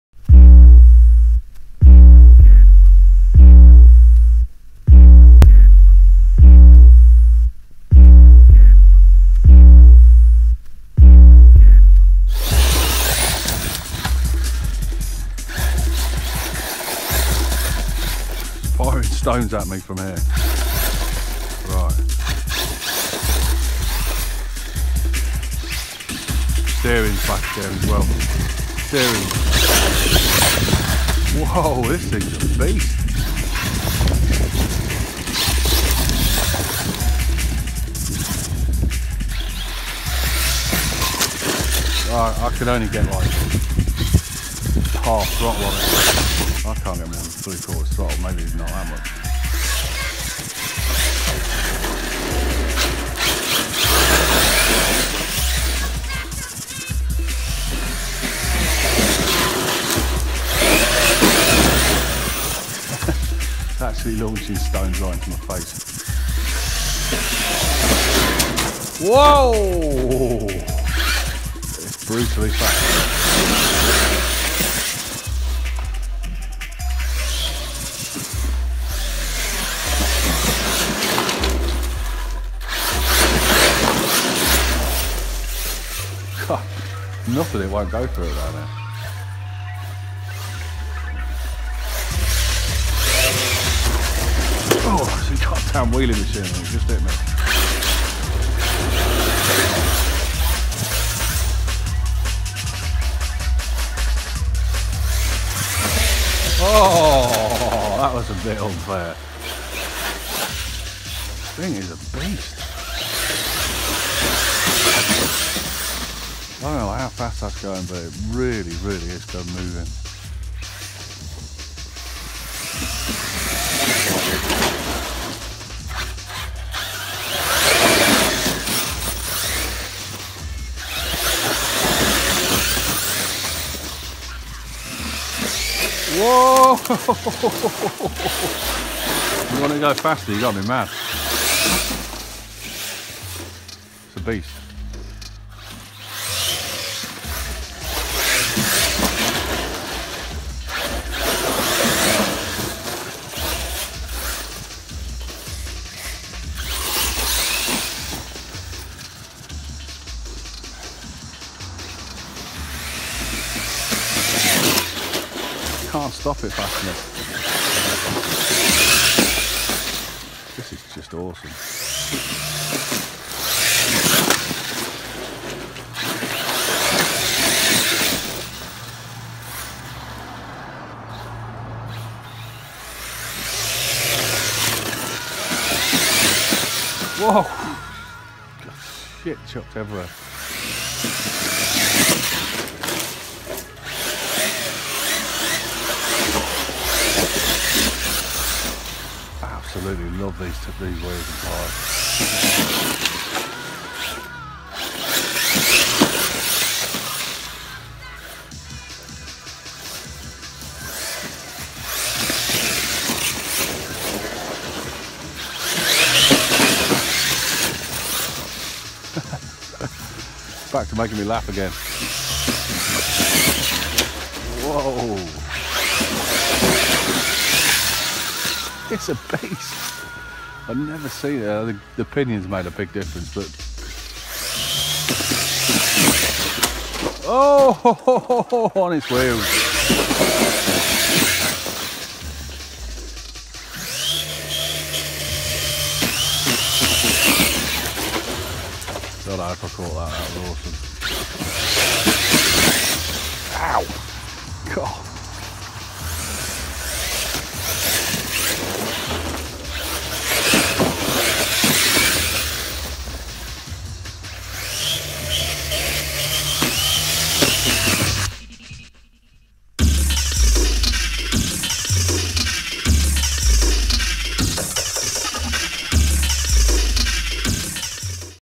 Arrma Kraton 6s railway bash, no chitter chatter, just blasting
Short version of the railway bash, less of me and more of the bashing